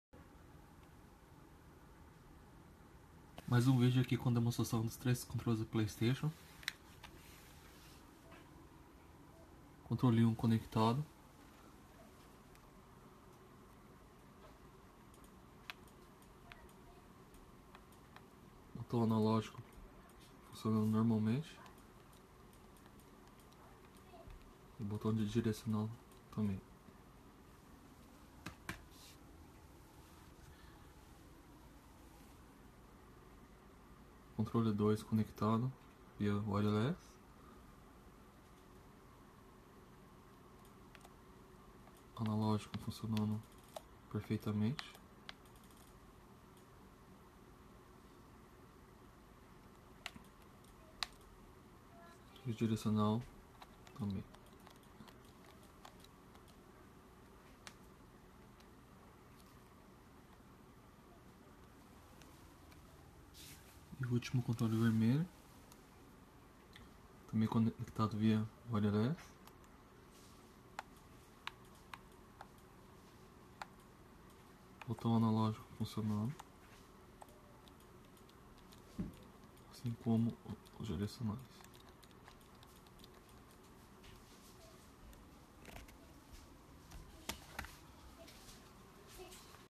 3 Controles do Playstation Video 2/2 Vendido
ATENÇÃO TODOS CONTROLES TESTADOS E FUNCIONANDO FAVOR ASSISTIR O VIDEO

Controle unitário valor ¥1,500
Favor não insistir para desconto pois o preço ja esta barato 👍
Envio para qualquer lugar no Japão pagamento por depósito ou na entrega com taxa de ¥540
Envio por conta do cliente!!
Se inscrevam e receba sempre varios videos de produtos a venda!!!
Informações sobre o Produto ligar ou mandar mensagem inbox ou pelo número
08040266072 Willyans